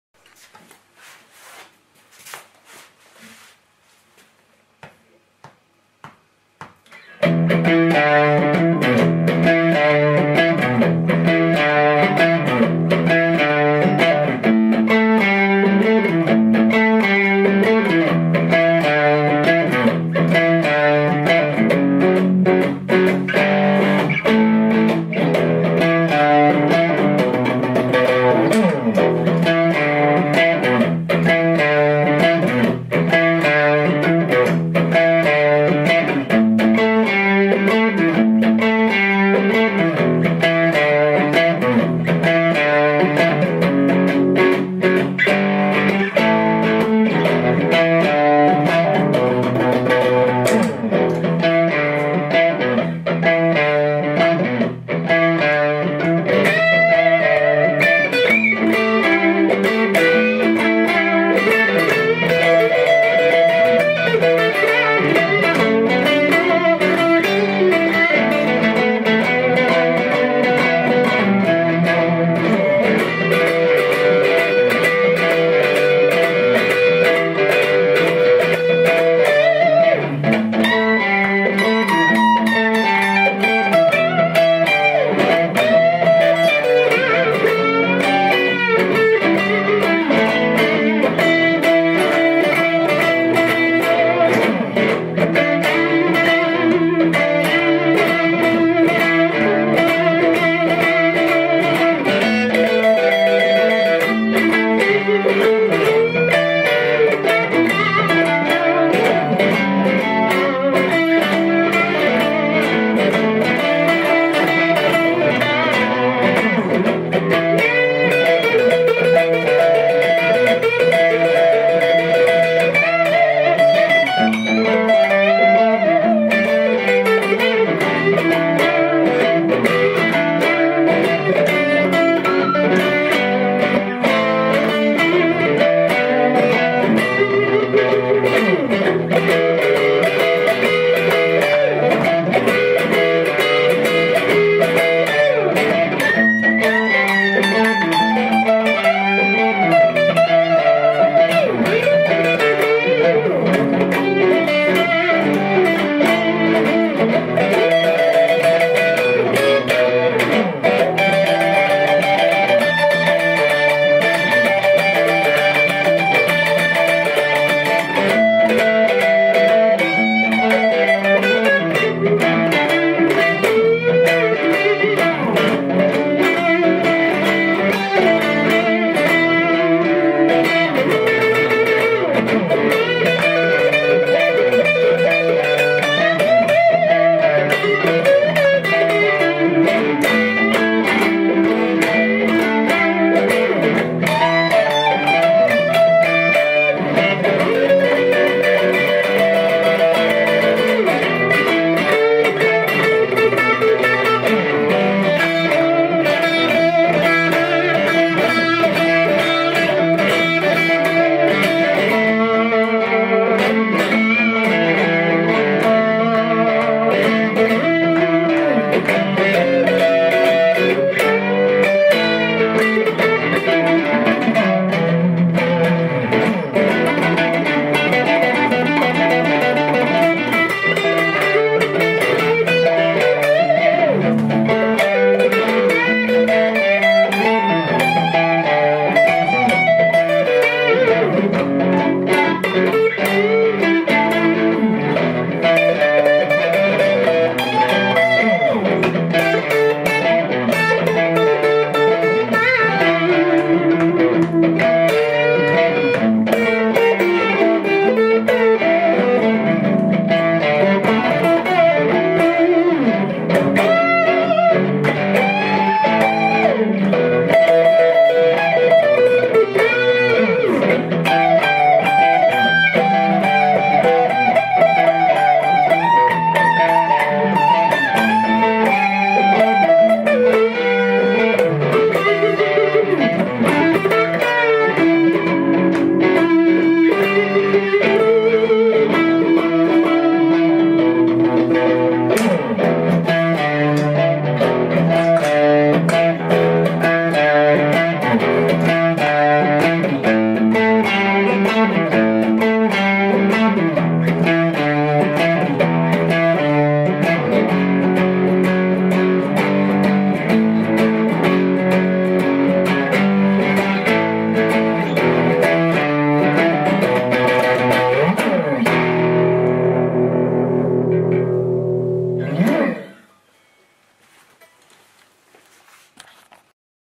Friday Blues Impro
As life is really good for me right now, I was thinking: why not publishing a video.
So direct take, recorded with cam recorder, so it is the direct sound.

Fender Clapton, Mesa Boogie TA-15, TS-9, and boss RC-3.